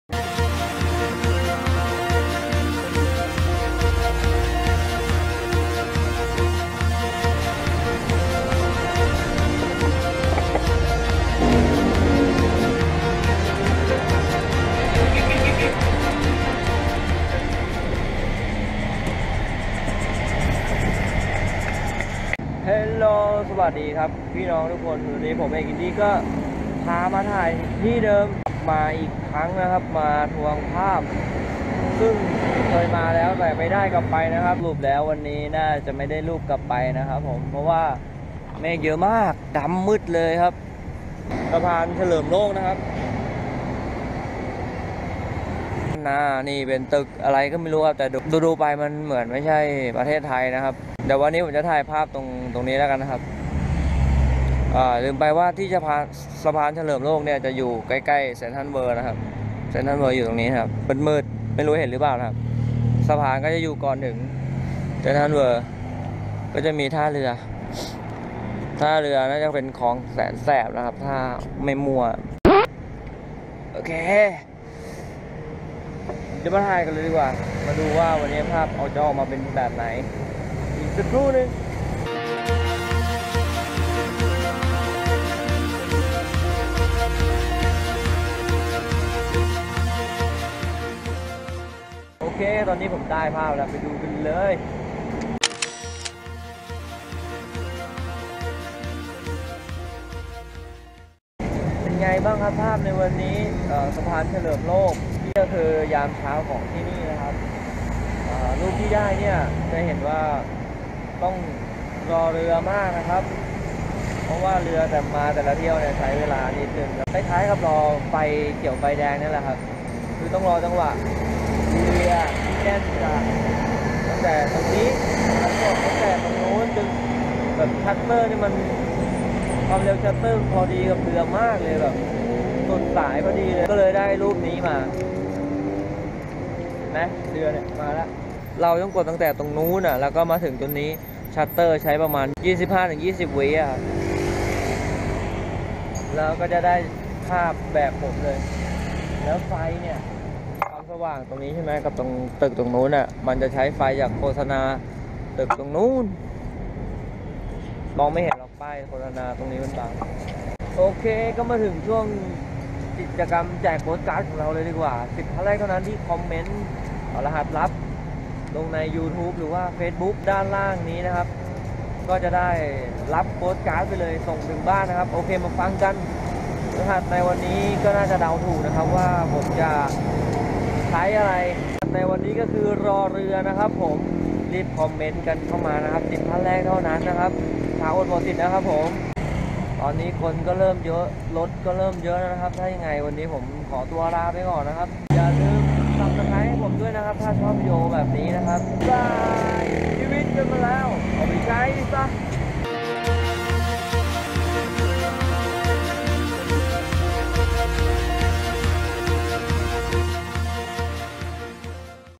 ถ่ายรูป สะพานเฉลิมโลก | Chalerm Loak Bridge | #22
Project : Sunrise 30 Day in Bangkok | Chalerm Loak Bridge
เป็นการฝึกถ่ายรูปพระอาทิตย์ขึ้นยามเช้า ของกรุงเทพฯ
30 วัน 30 สถานที่

วีดีโอนี้เป็นวันที่ 22 ของการถ่ายสถานที่ก็คือ สะพานเฉลิมโลก

ดูวีดีโอวันที่ 21 สะพานกรุงธน | Krung Thon Bridge

หรือตามผมได้ที่เฟสนี้